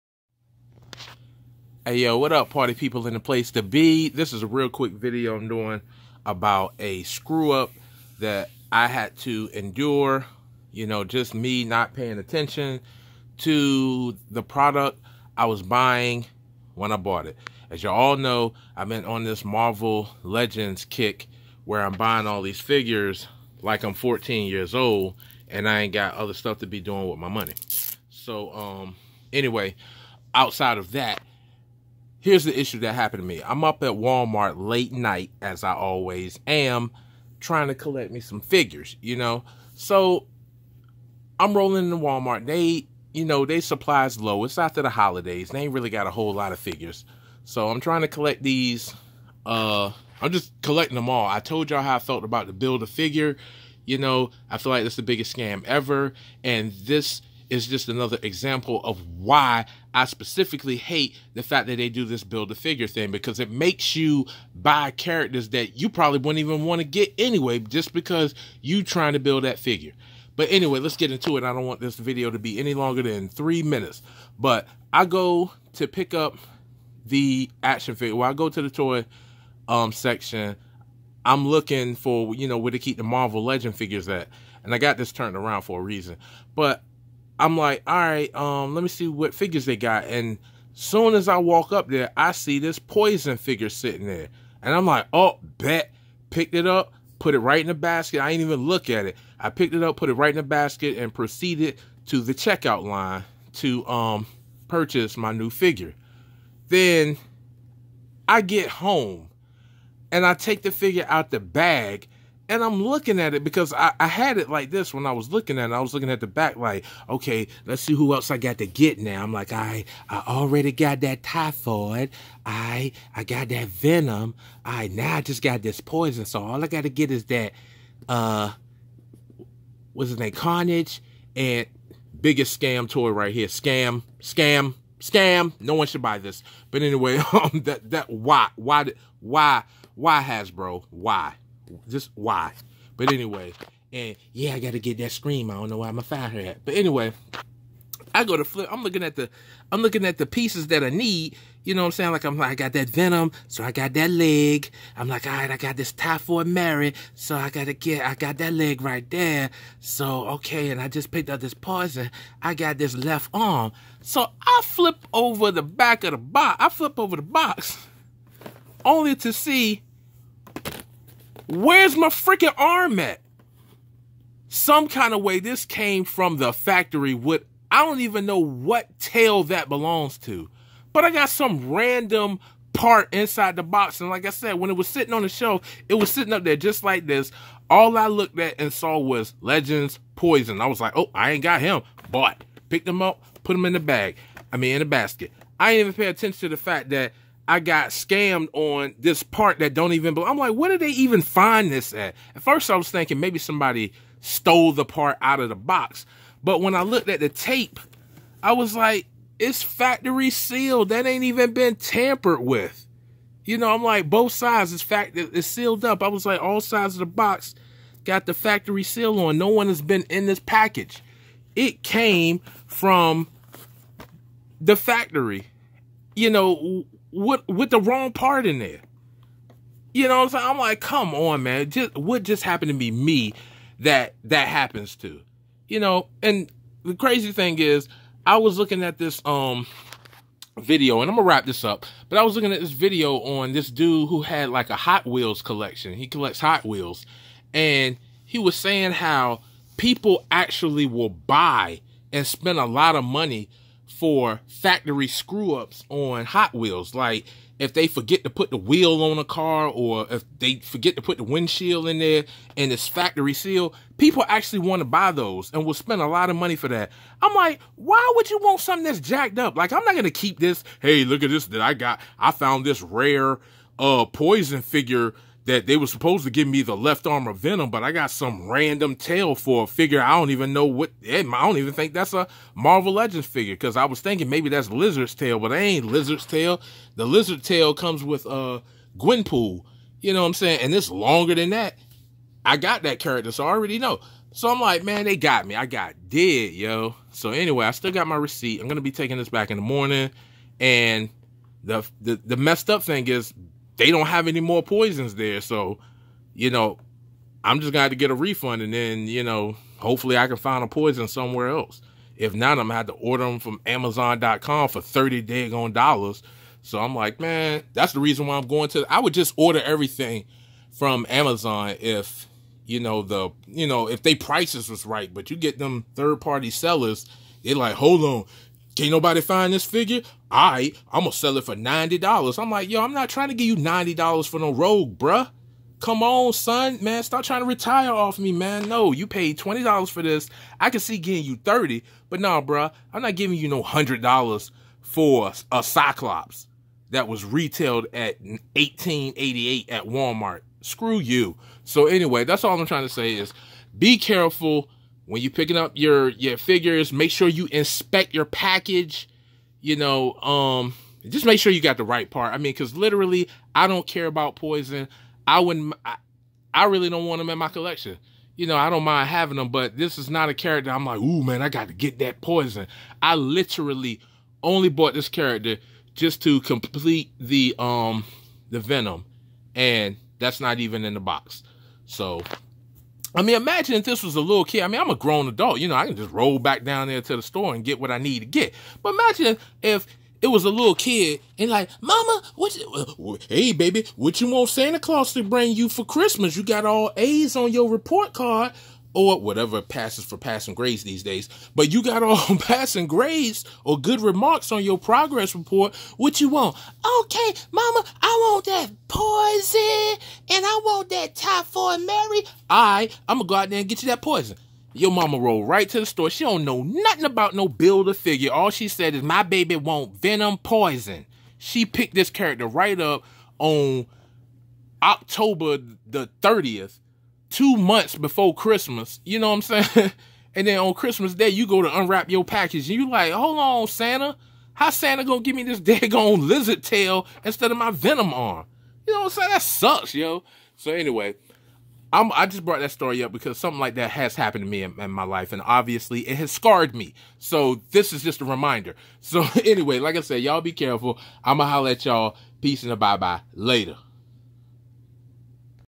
Marvel Legends Poison Figure BAF DISASTER
This is what happens when you don't pay attention to what you're buying and also why I HATE Hasbro's BAF gimmick. Just sell the figures separate and stop compelling customers to buy figures they don't really want just they don't have extra body parts laying around already!